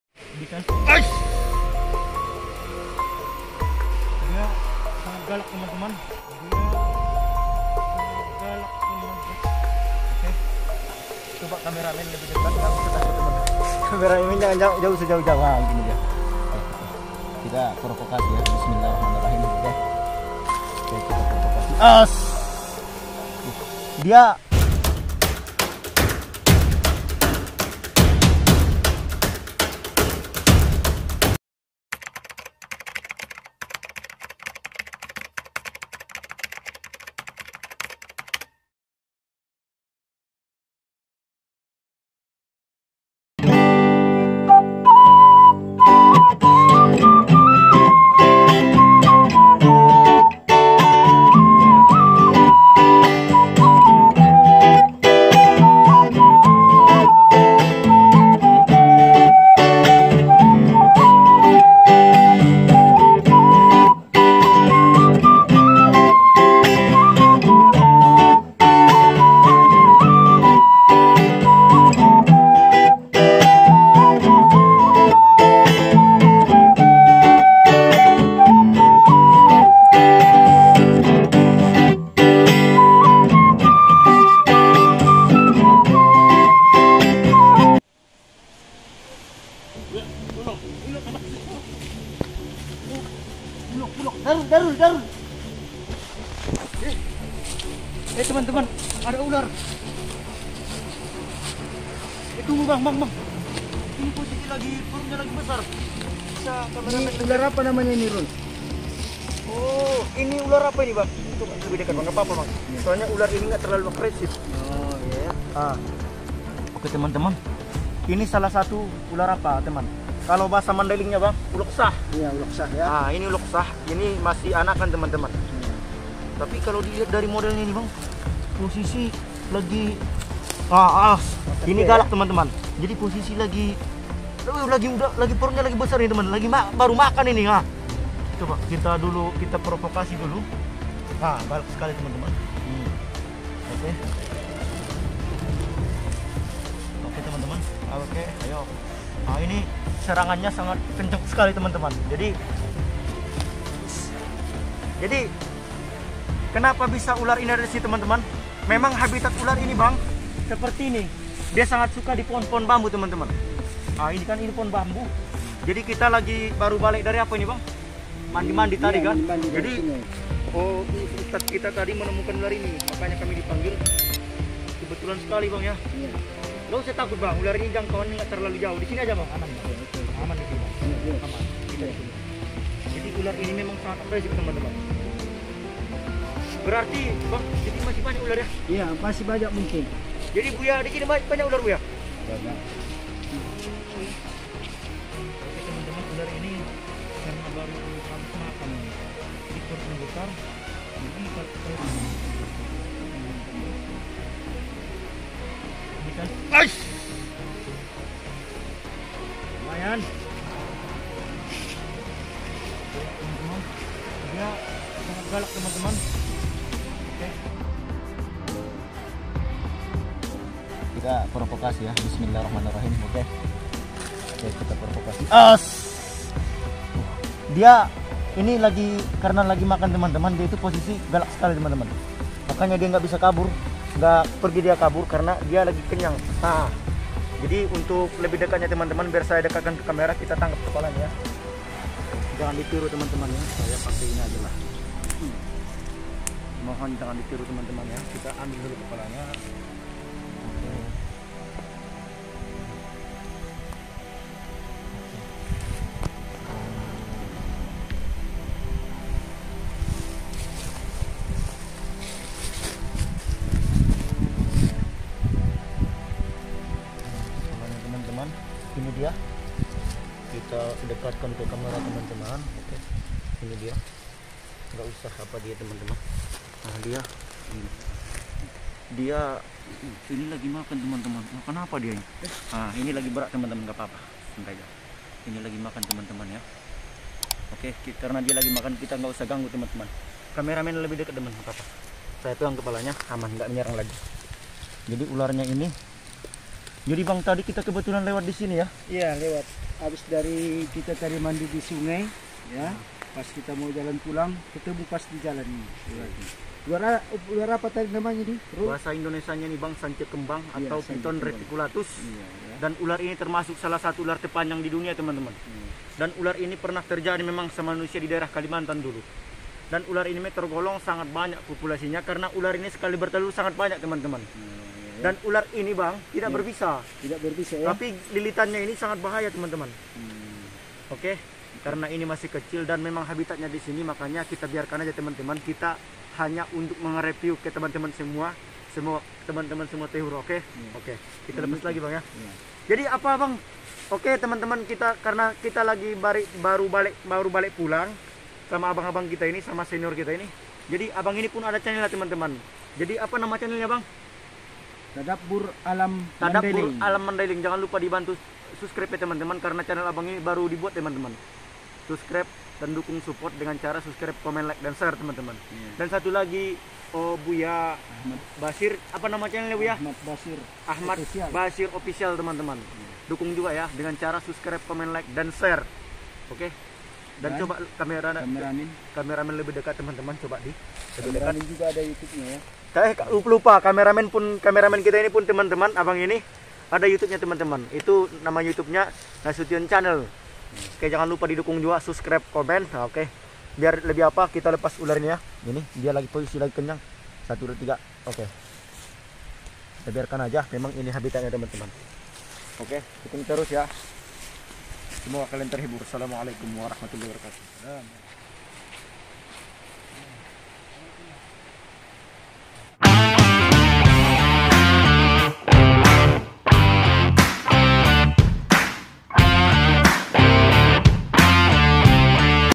Jalan Pulang Jumpa Ular Lagi Menelan Mangsa ‼️
disaat pulang dari sungai tiba tiba kita melihat ular lagi menelan mangsa dan kondisi perutnya sangat besar..
dan kita penasaran untuk itu ikuti keseruan kita teman teman ..
terimakasih...😊😊😊

Suppot Channel...